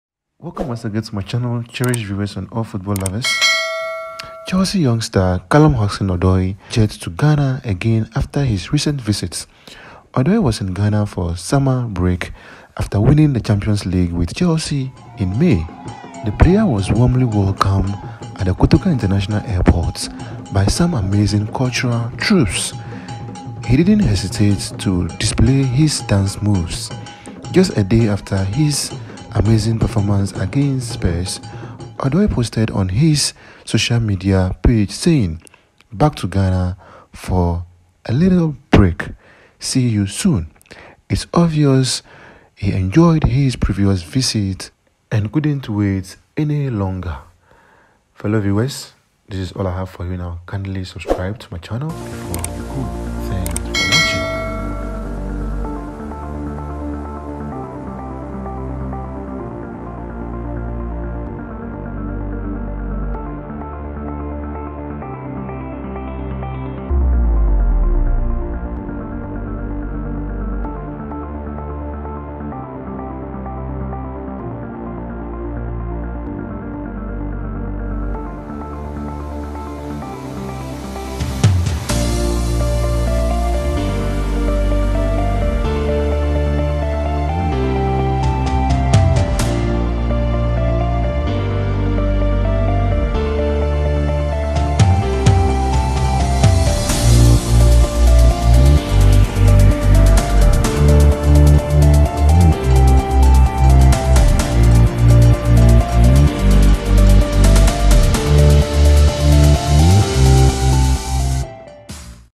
Welcome Home StarBoy!🔥Callum Hudson-Odoi jets🛫 off to Ghana 🇬🇭 for a short break!
Callum Hudson-Odoi jets to Ghana for a short break!